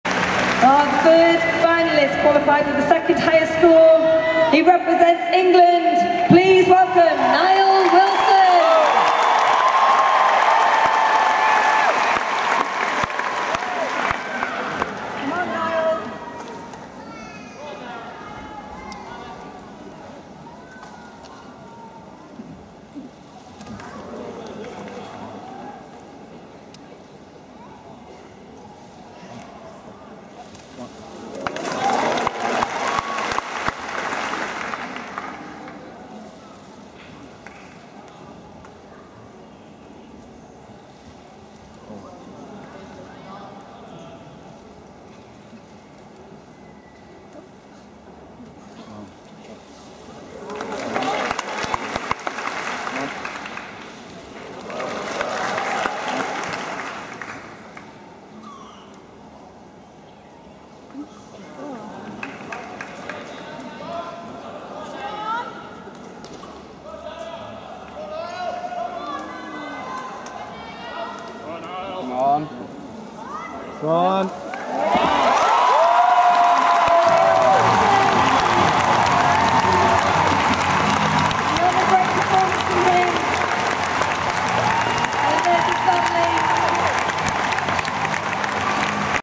Nile Wilson (ENG) - High Bar - 2014 Commonwealth Games (Finals)
14.966
1st Place